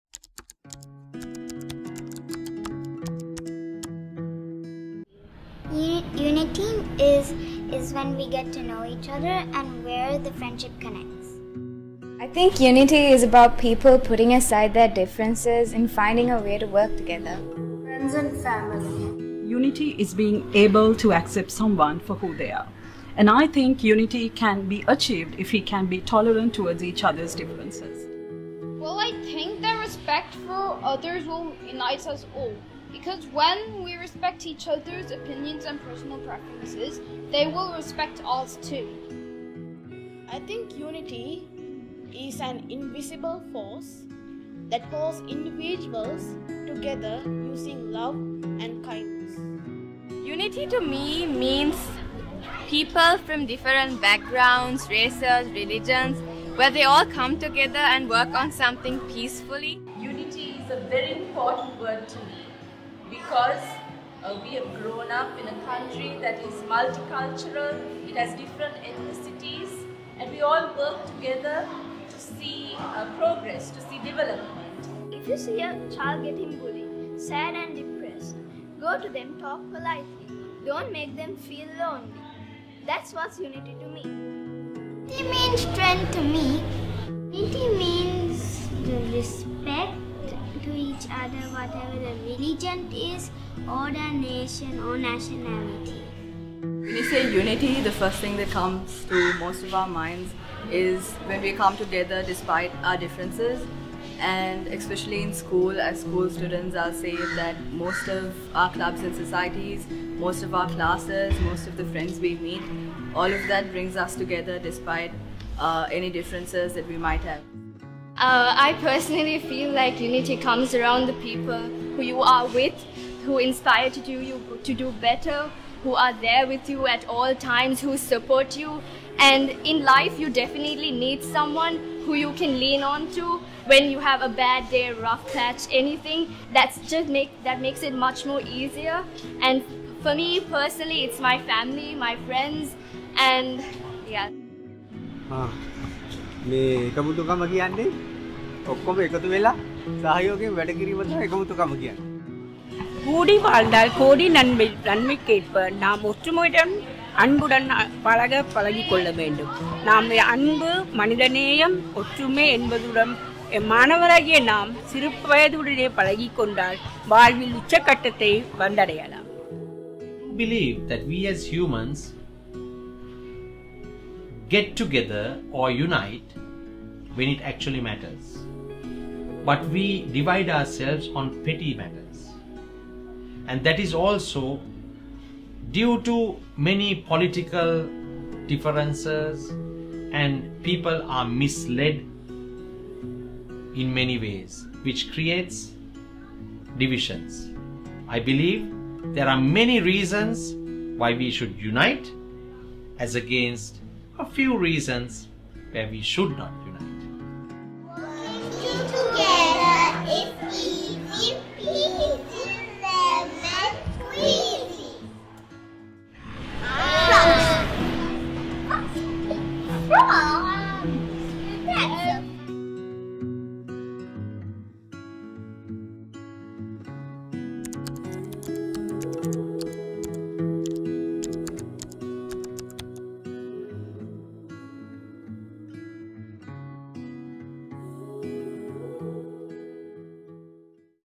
Unity - by the Film Club of Wycherley International School
The students of the newly inaugurated Film Club, produced their first documentary - "Unity" as the pilot project, after the completion of a 3 week youth filmmaking programme -CAMARA CHICA organized by British Council, delivered in collaboration with UK partner Into Film and in-country partner organizations.